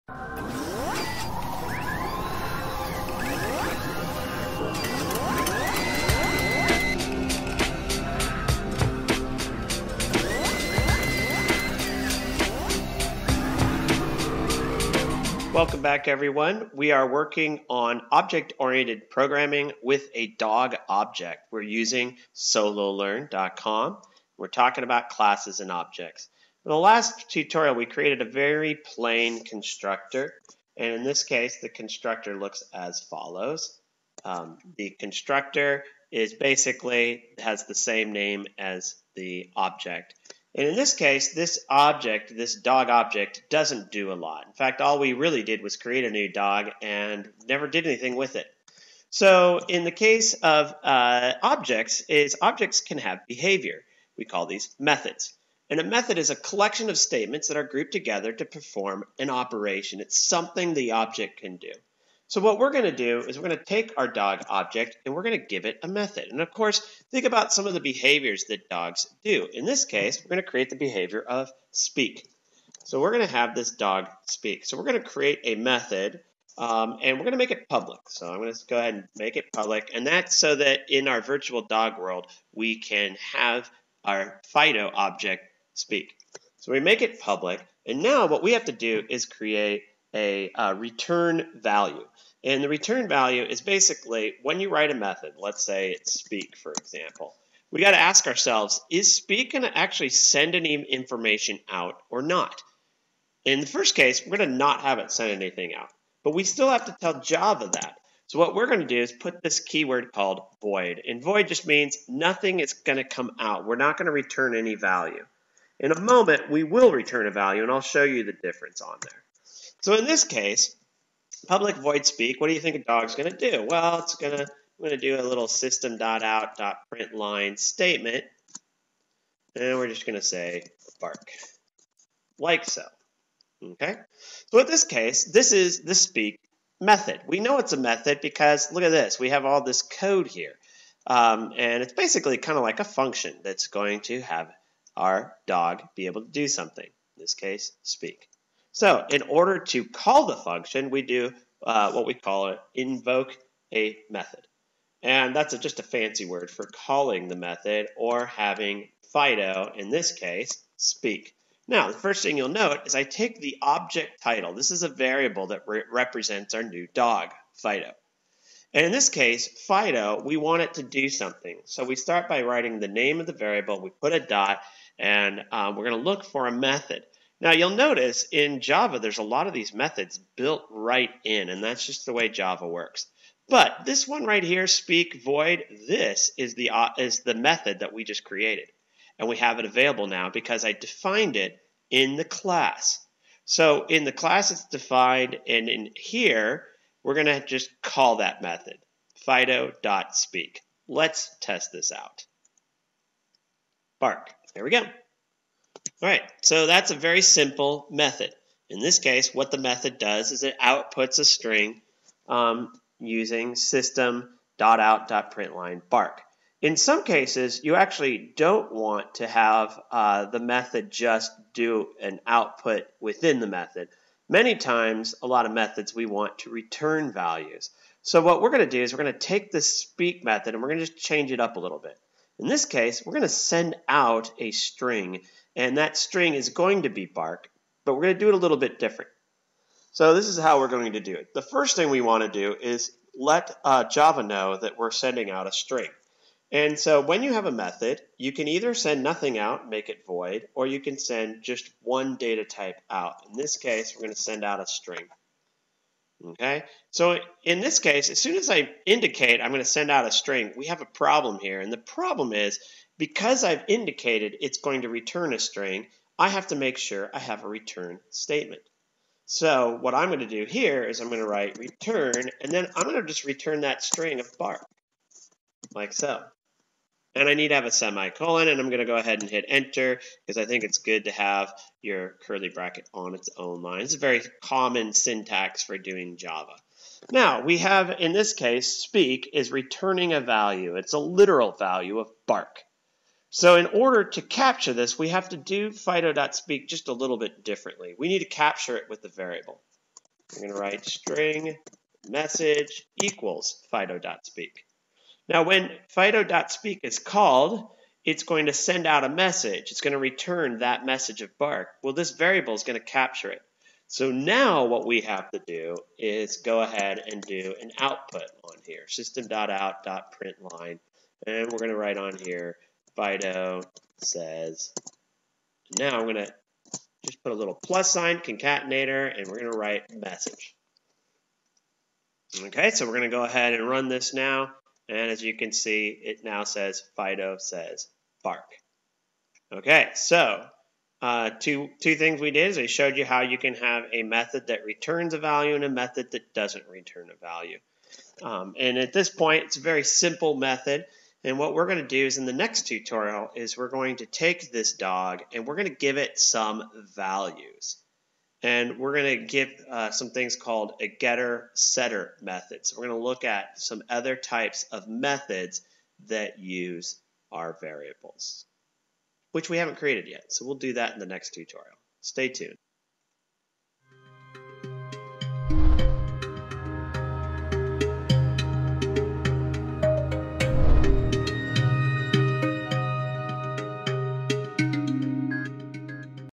SoloLearn Java Objects Part 2 - Give Your Dog Behavior
Part 2 of my mini-series on Java Objects from SoloLearn. In this video, we give our Dog a speak() method. I show you two versions of the method. In the first version, we use simple output (System.out.println()), but then we change the method to return a String.